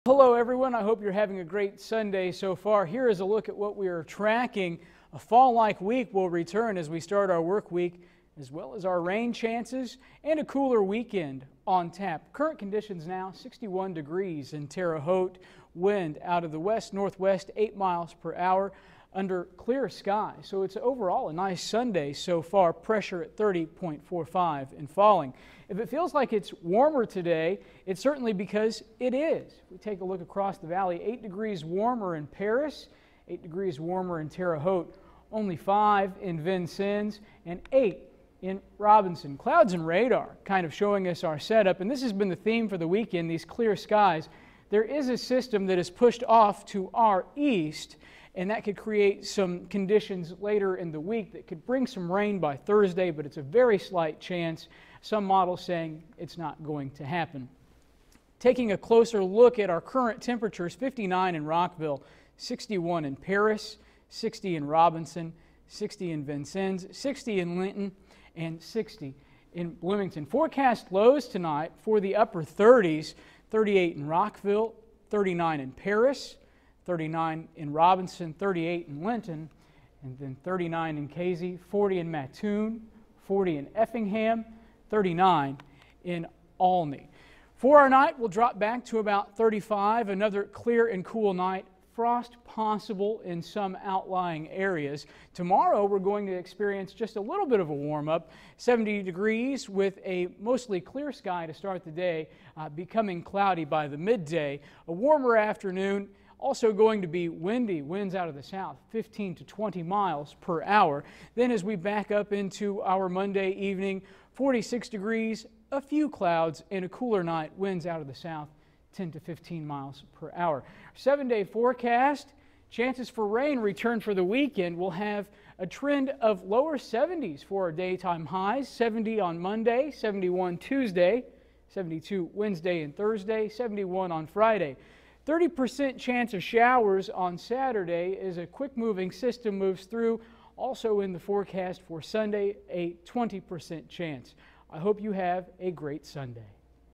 Sunday Early Forecast
More dry days and a slight warming trend